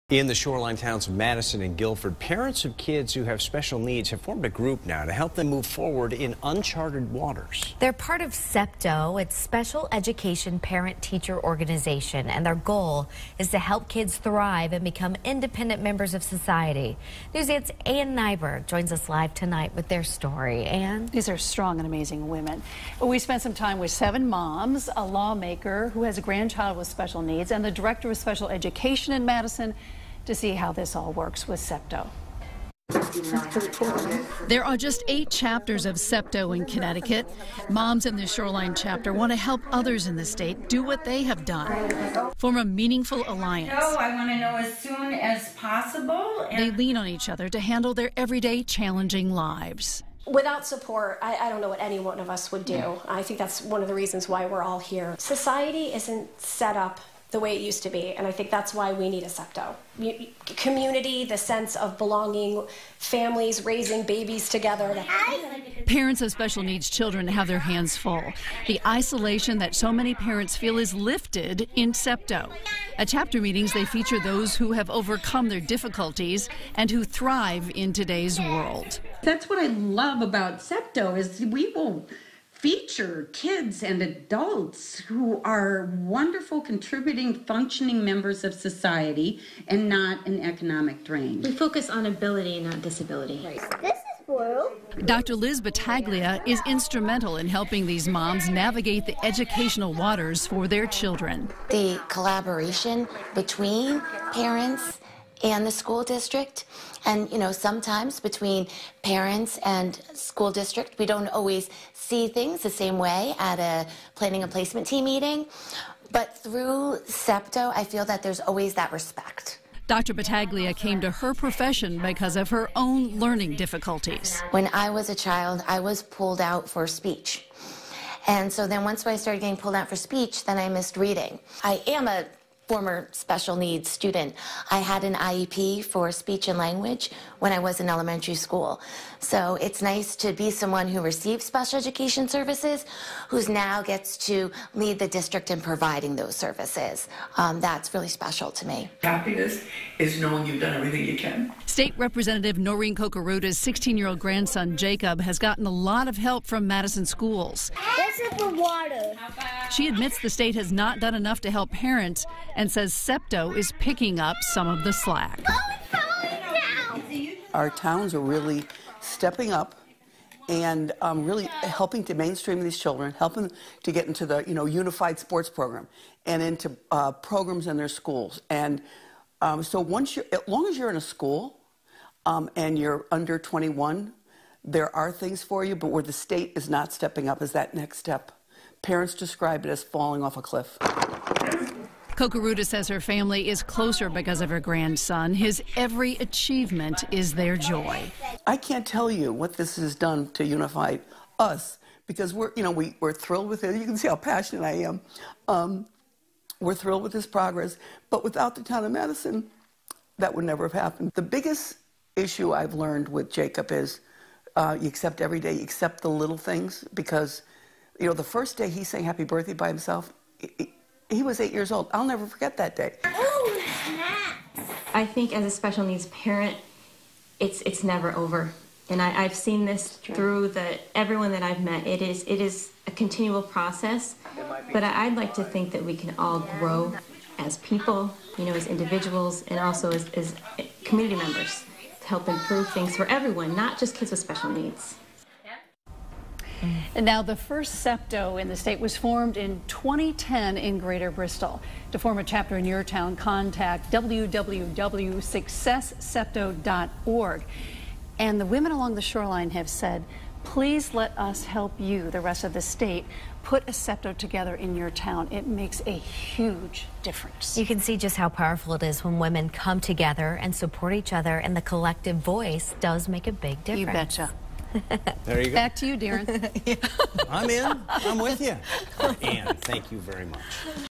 Group helps support shoreline moms with special needs children
There are just eight chapters of SEPTO in Connecticut. Moms in this shoreline chapter want to help others do what they've done, which is form a meaningful alliance.